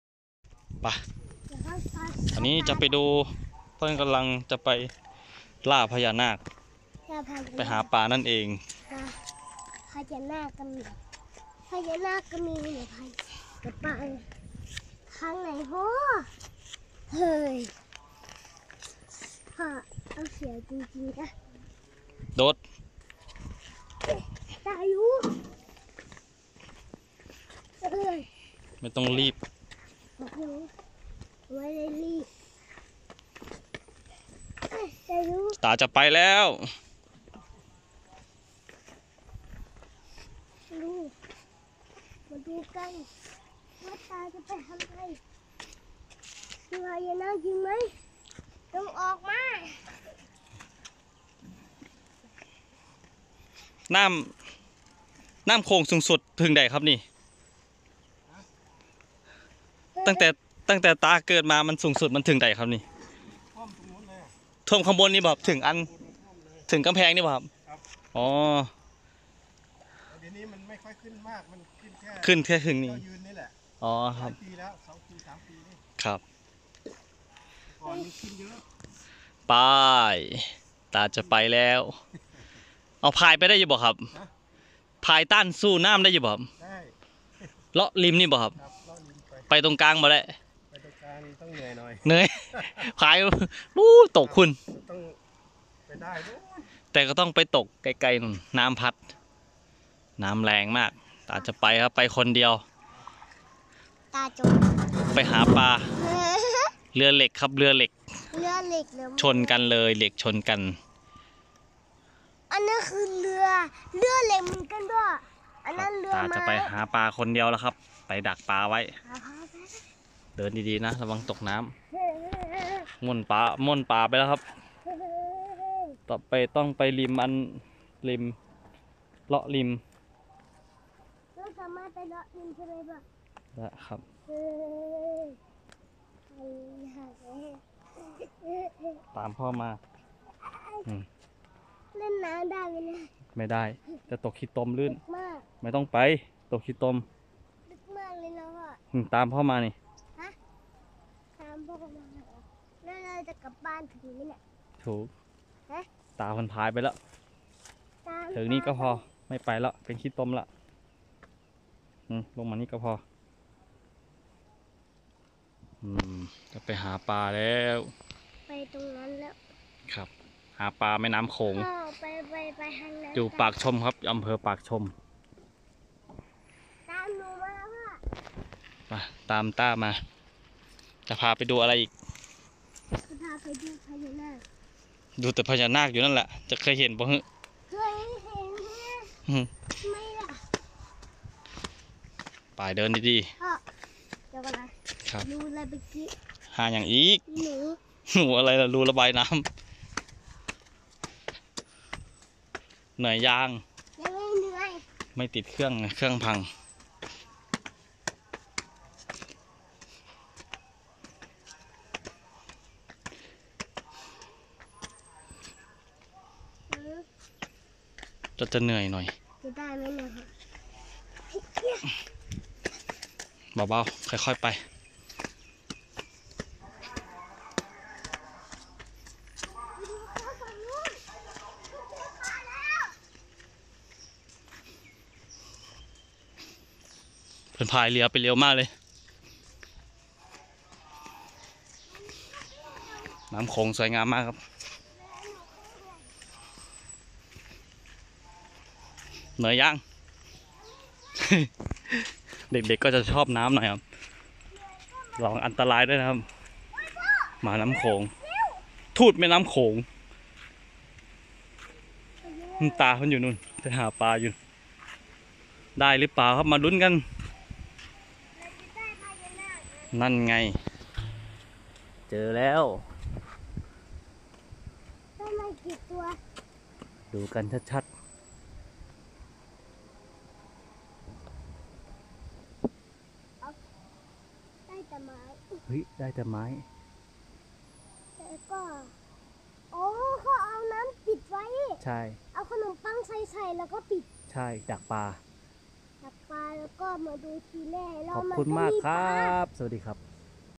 คุณตาจะไปหาปลาแม่น้ำโขงครับ ที่อำเภอปากชม
คุณตาจะไปหาปลาแม่น้ำโขงครับ ที่อำเภอปากชม@user-MeechookThailand